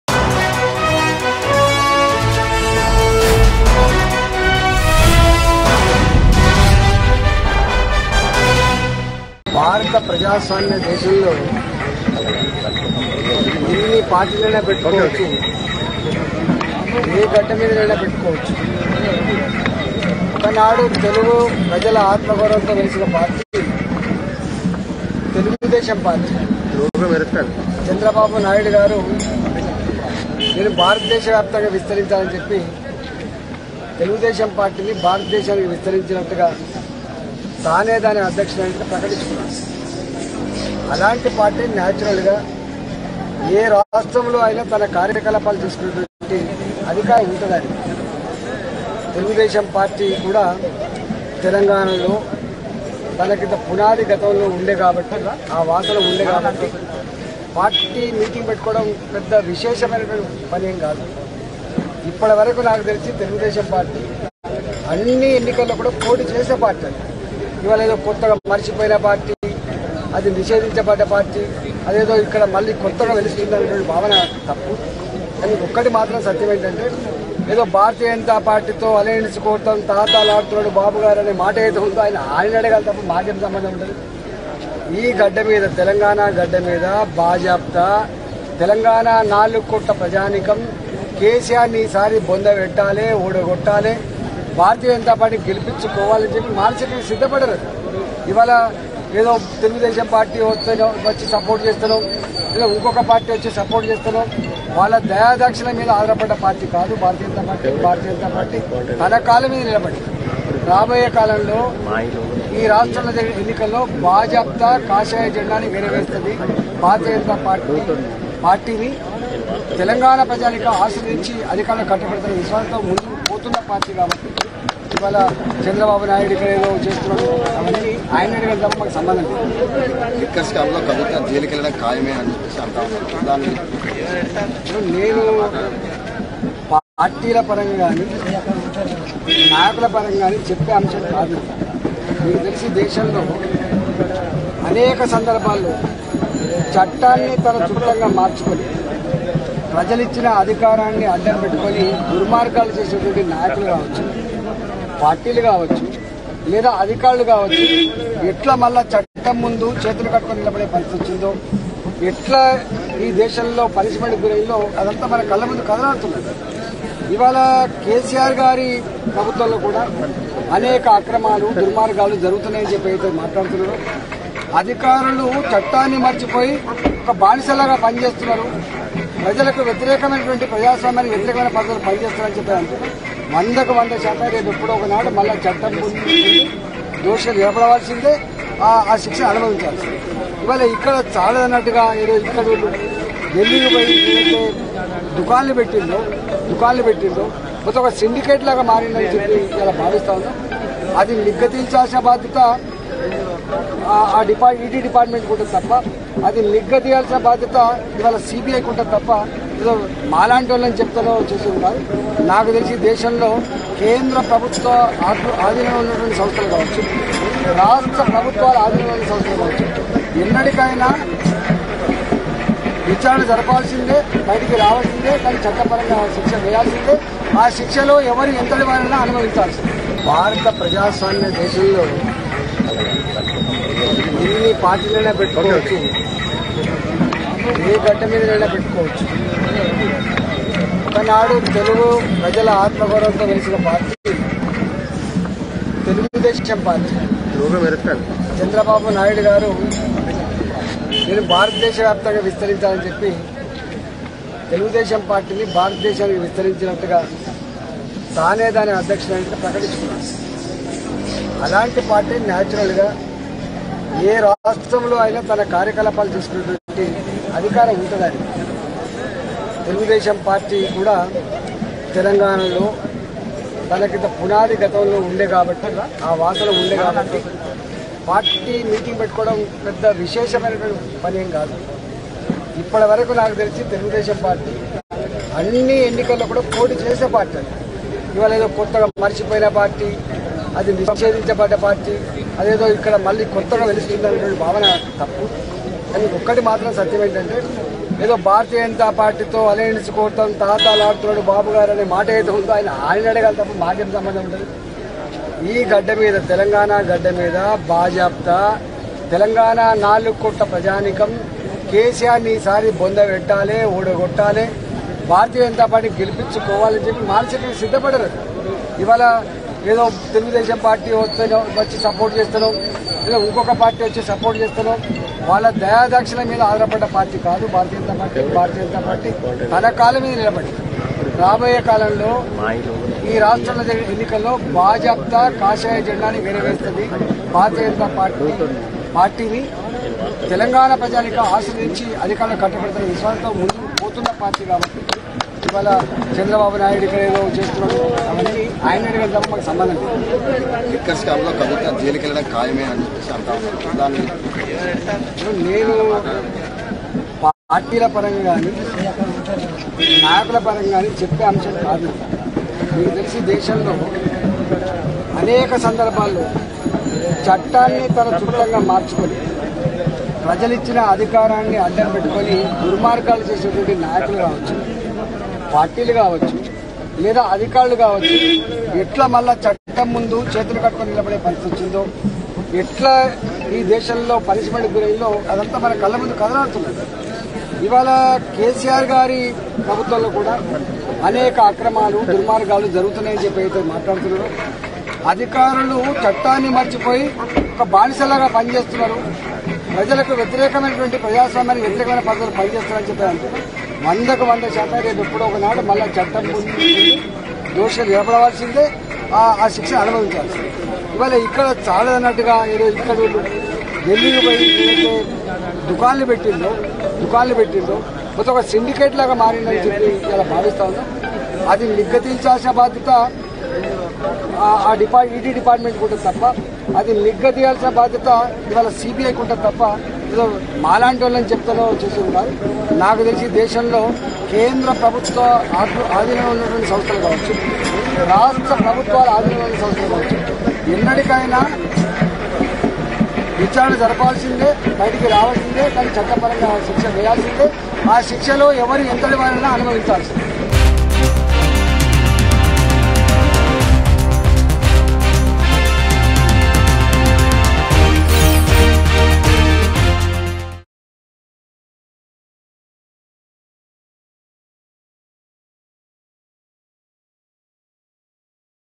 BJP MLA Etela Rajender Interesting Comments On Chandrababu Naidu Khammam Meeting | Mango News
BJP MLA Etela Rajender Interesting Comments On Chandrababu Naidu Khammam Meeting.